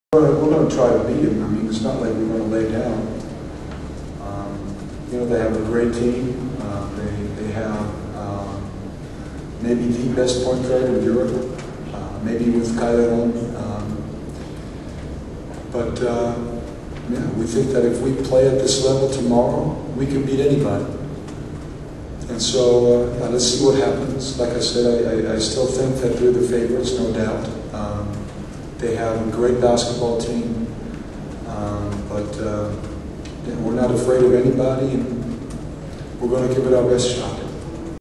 Bauermann: "We gonna beat Lithuania"
German basketball coach is ready to fight against Lithuania in Eurobasket2007.